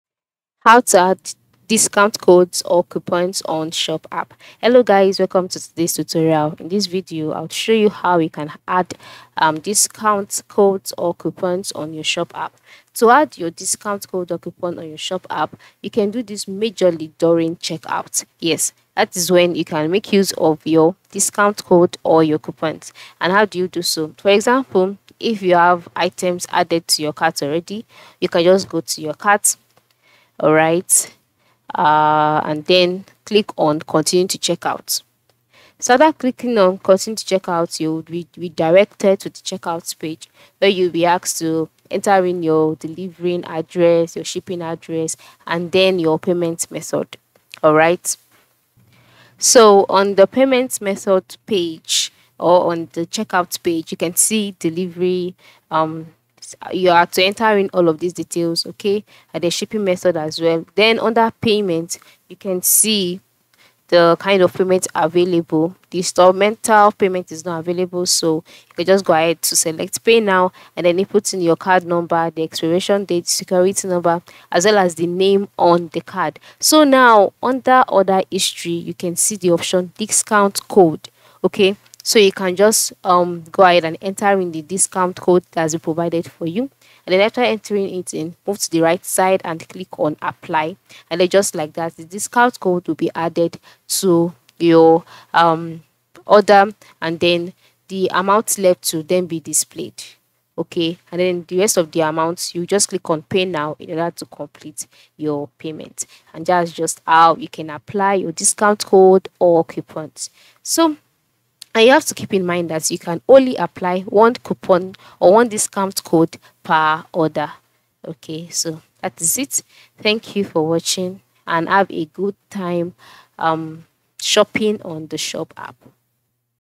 How To Add Discount Codes or Coupons on Shop App (Quick Tutorial)
In this video I will show you How To Add Discount Codes or Coupons on Shop App. It would be good if you watch the video until the end so that you don't miss important steps.